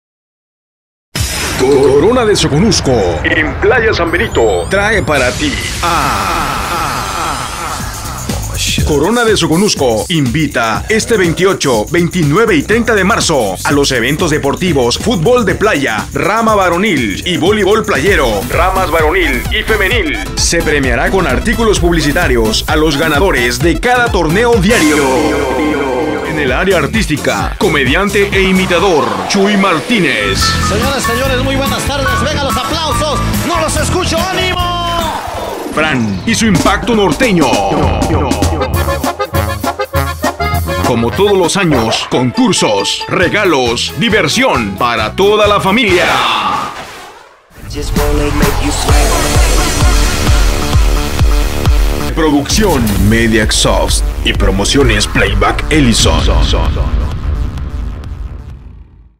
[HD] Semana Santa en Playa San Benito 2013
Corona de Soconusco te invita a San Benito a disfrutar en esta Semana Santa 2013 de todo lo que tiene preparado para tí.